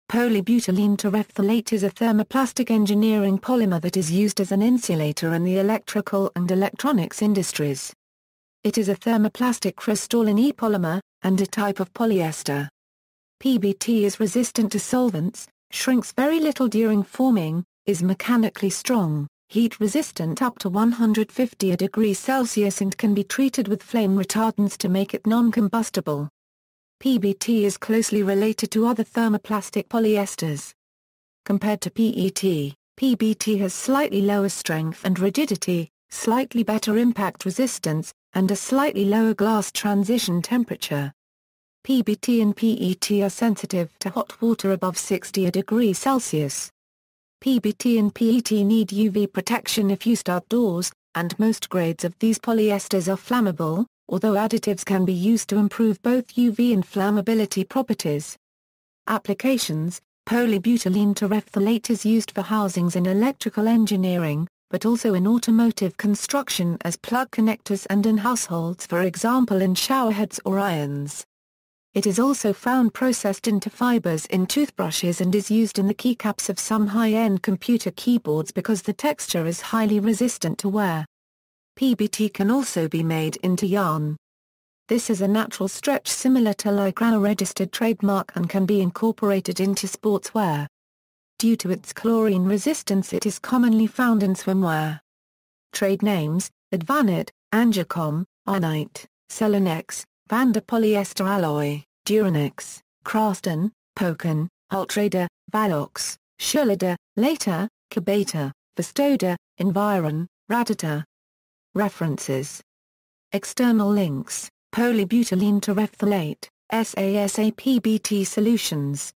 Polybutylene terephthalate
Polybutylene terephthalate is a thermoplastic engineering polymer that is used as an insulator in the electrical and electronics industries. It is a thermoplasticcrystalline polymer, and a type of polyester. PBT is resistant to solvents, shrinks very little during forming, is mechanically strong, heat-resistant up to 150 °C and can be treated with flame retardants to make it noncombustible.
PBT is closely related to other thermoplastic polyesters. Compared to PET, PBT has slightly lower strength and rigidity, slightly better impact resistance, and a slightly lower glass transition temperature. PBT and PET are sensitive to hot water above 60 °C. PBT and PET need UV protection if used outdoors, and most grades of these polyesters are flammable, although additives can be used to improve both UV and flammability properties.